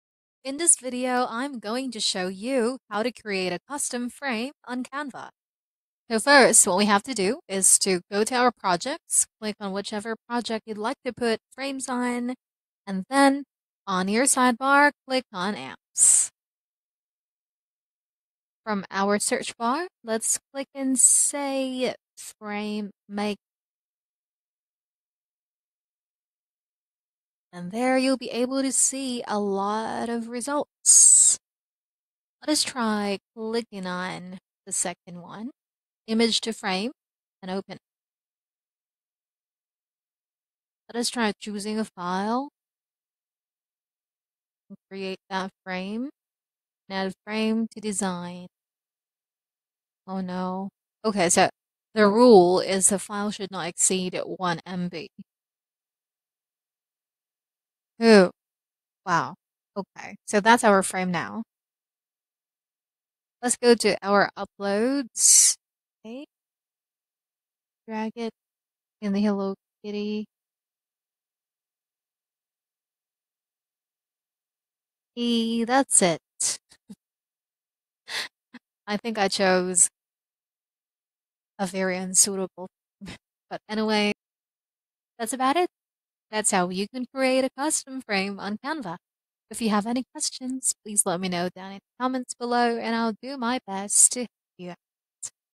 How to Create a Custom Frame on Canva [QUICK GUIDE]
In this video, I'm going to show you how to create a custom frame on Canva.